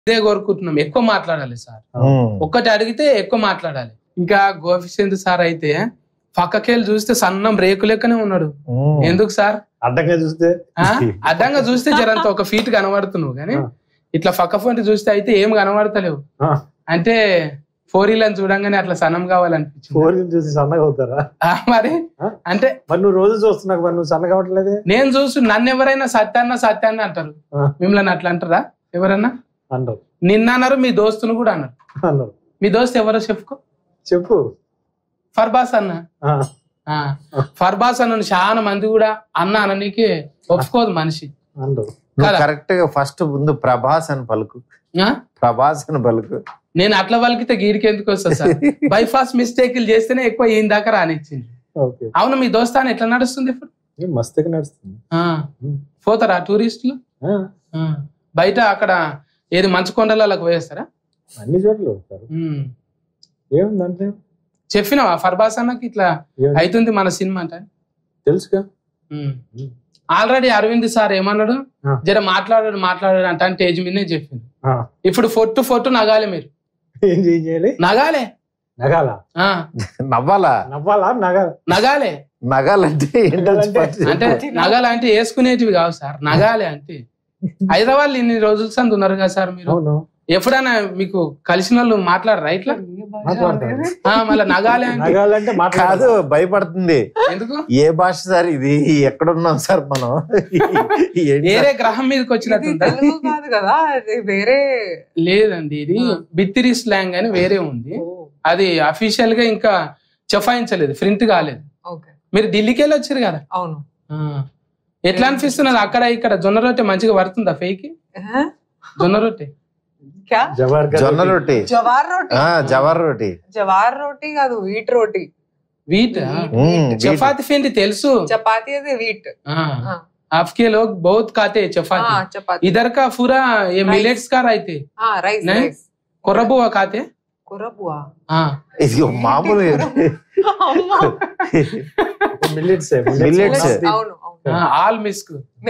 Bithiri Sathi Funny Comments On Prabhas & Gopichand | Raashi Khanna, Maruthi | TT
Bithiri Sathi Funny Comments On Prabhas & Gopichand | Raashi Khanna, Maruthi